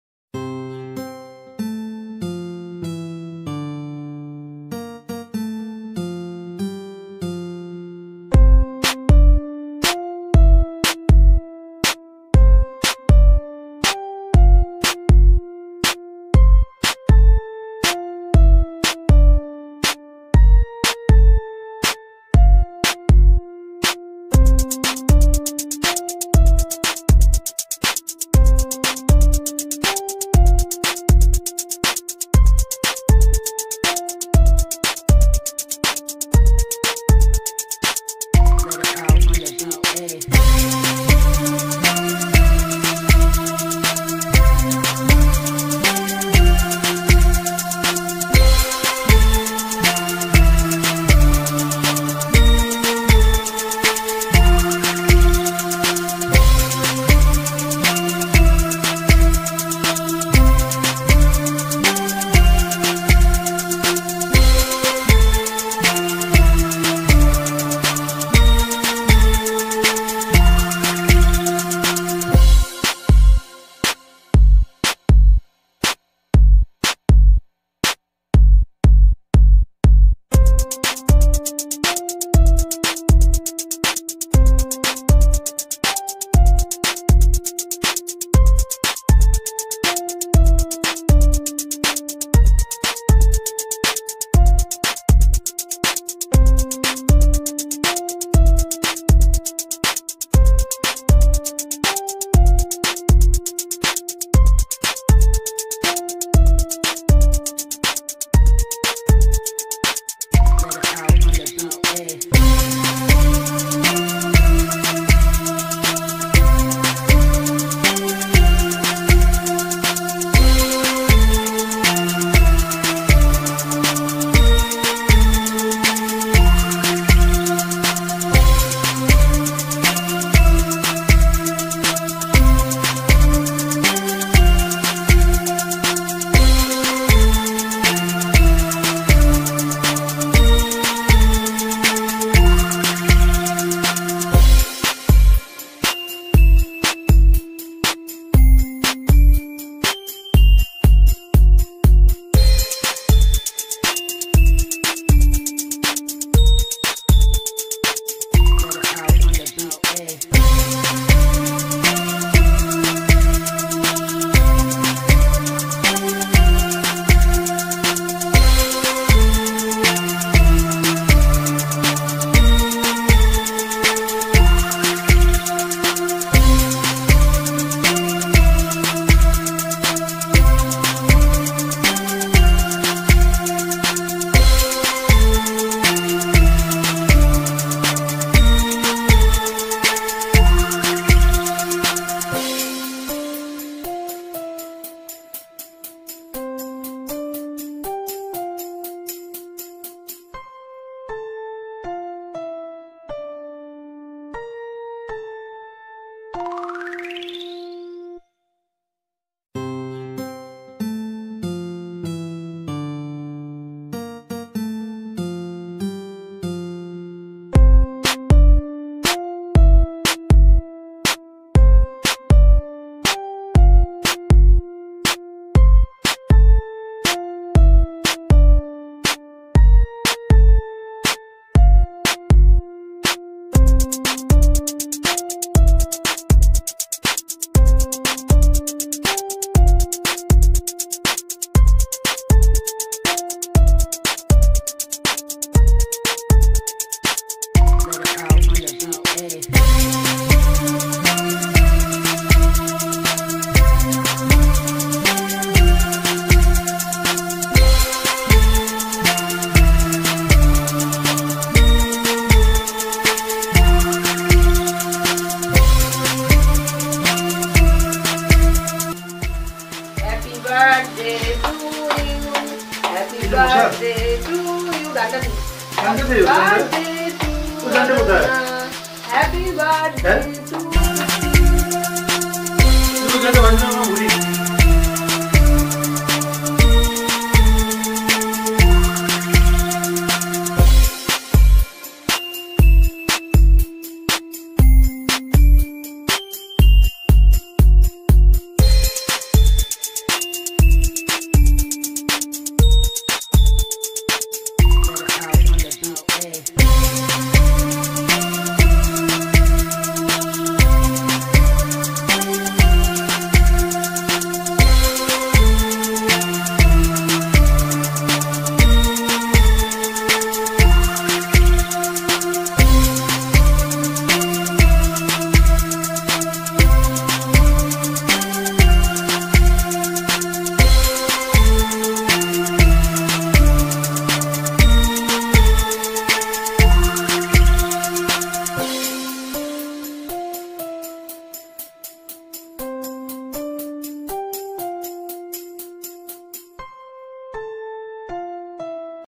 LATENIGHT BIRTHDAY WISH FROM MAA & BAPI
NIGHT WISH OF MY PARENTS ON MY BIRTHDAY..
EVENNG PARTY IS LEFT
TOMORROW PARTY VIDEO WILL E UPLOADED..
MY 25TH BIRTHDAY CELERATION